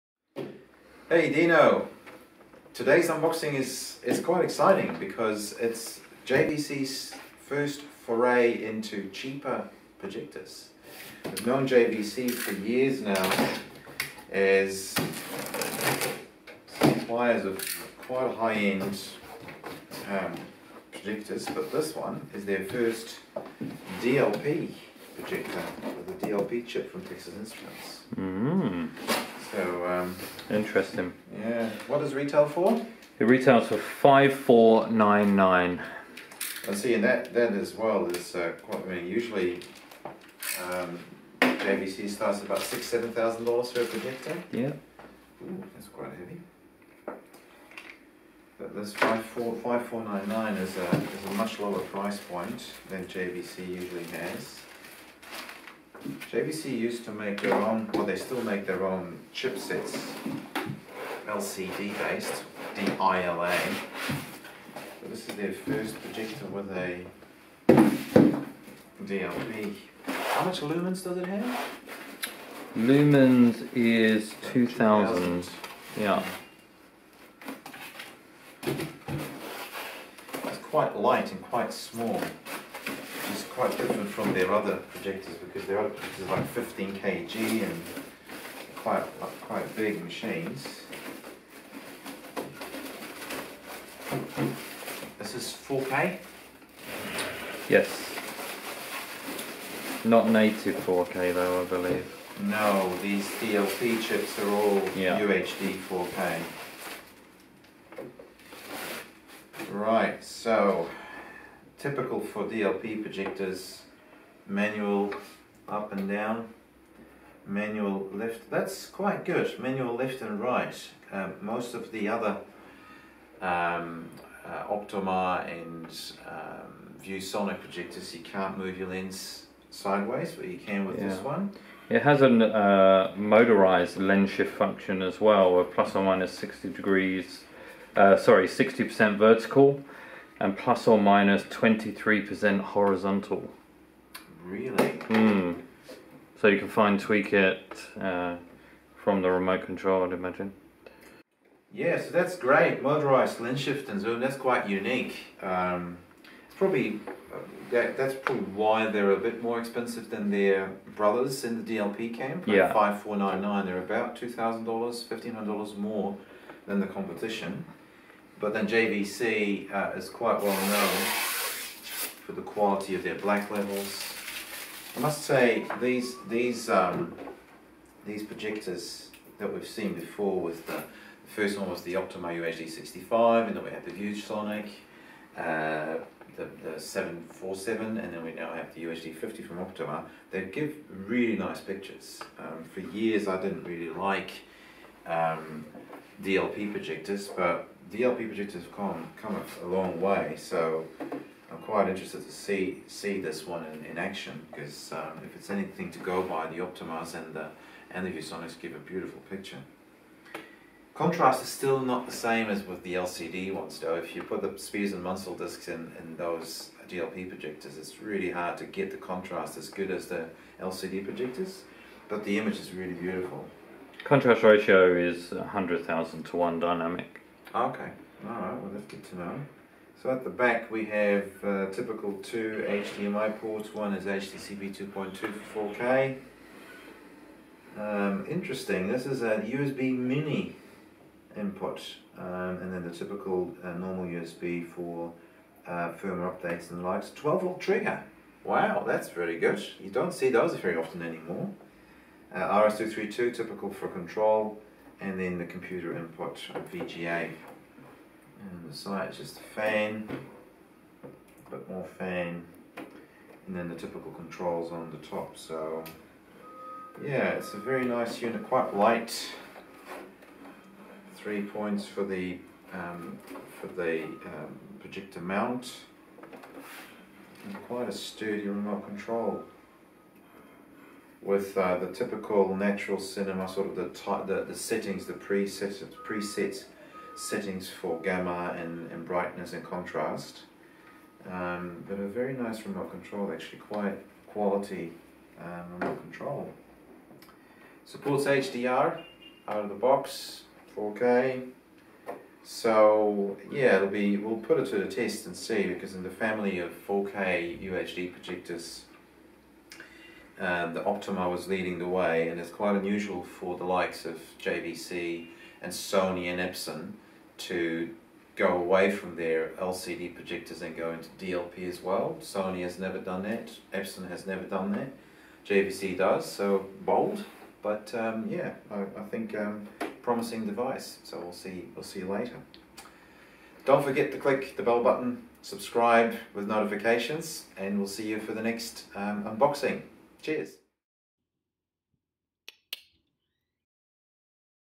JVC LX-UH1 4K UHD/HDR Projector Unboxing
Technical Specifications:

Device: 0.47" DMD (1920 x 1080)
Resolution:  3840 x 2160
Lens shift function:  +/-60% vertical & +/-23% horizontal, motorized
Light source lamp: NSH 240 W
Lamp life: Approx. 4,000 hours in normal mode, 10,000 hours in eco mode
Brightness: 2,000 lm
Contrast Ratio: Dynamic: 100,000:1
Video input terminals (back panel):
PC
1 (D-sub 15pin)
Control terminals
RS232C
1 (D-sub 9-pin)

General
Noise level: 29 dB (Eco)/ 33 dB (Normal)
Power requirement: AC110 V-240 V, 50/60 Hz
Dimensions: W x H x D, mm: 333 x 135 x 331.5 mm
Dimensions: W x H x D, inches: 13-1/8 x 5-5/16 x 13-1/16
Weight, lbs: 10.56